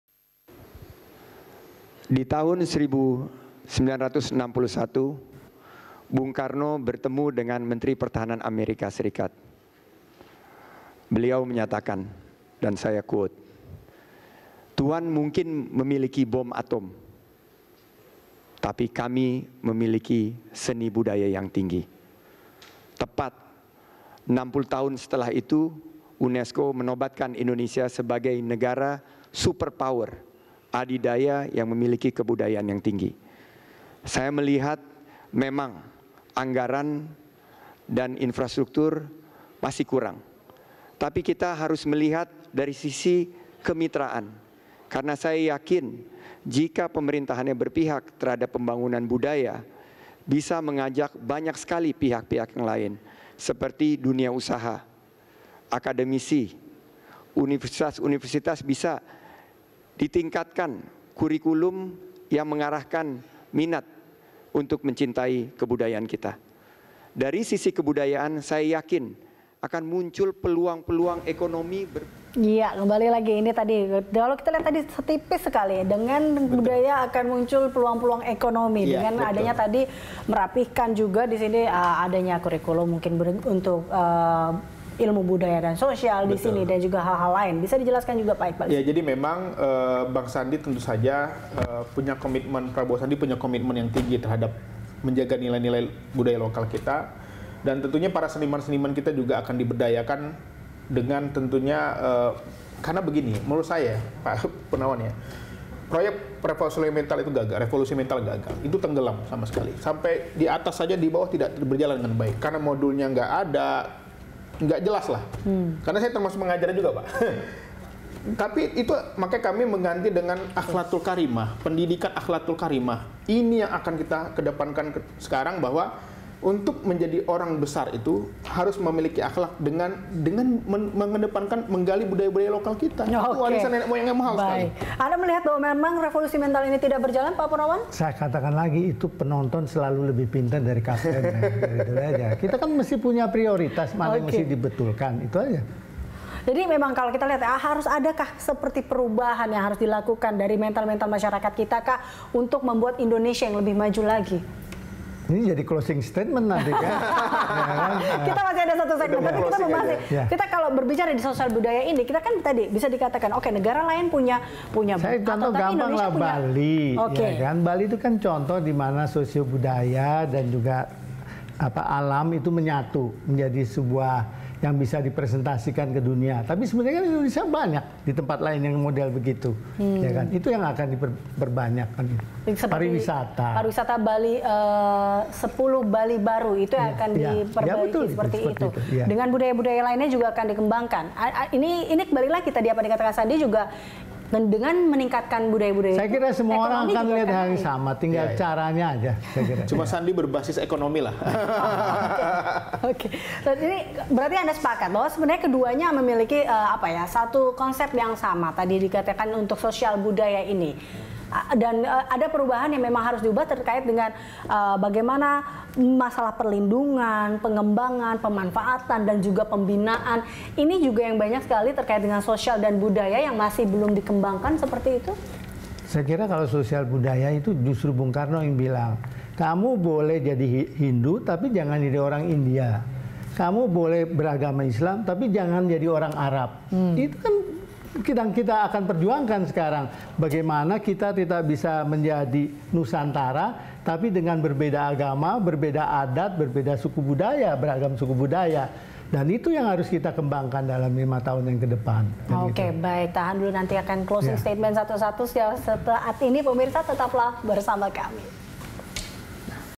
Revolusi Mental Tidak Berjalan, BPN: Kita Ganti dengan Akhlakul Karimah
Gerakan revolusi mental, menurut Juru Bicara BPN Prabowo-Sandi M. Iqbal tidak berjalan mulus. "Maka kita mengganti dengan pendidikan akhlakul karimah. Ini yang akan kita kedepankan," ucap Iqbal di Jakarta, Ahad (17/3/2019).

Facebook.com/BeritaSatu
Youtube.com/BeritaSatu